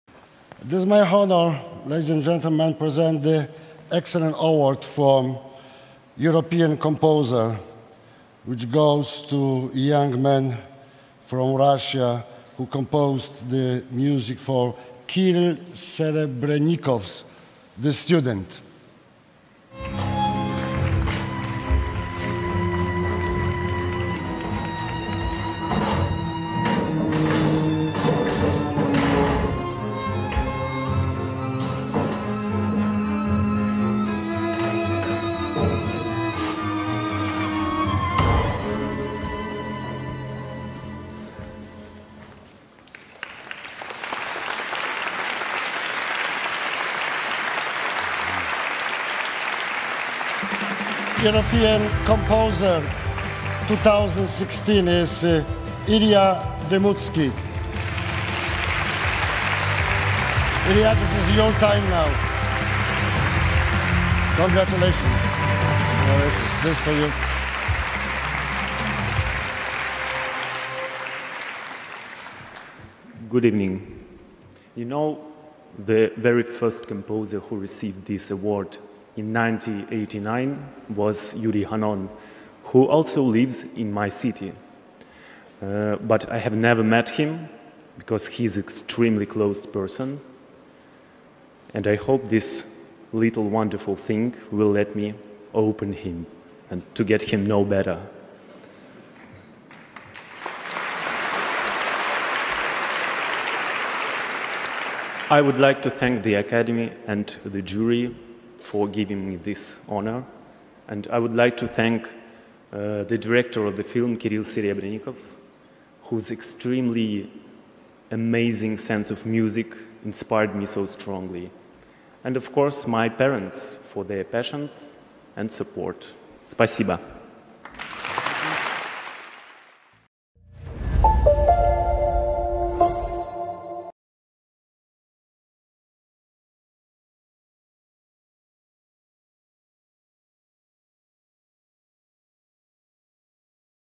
Ilya Demutsky - European Composer 2016 for THE STUDENT
Ilya Demutsky - European Composer 2016 for THE STUDENT by Kirill Serebrennikov at the 29th European Film Awards in Wroclaw, Poland.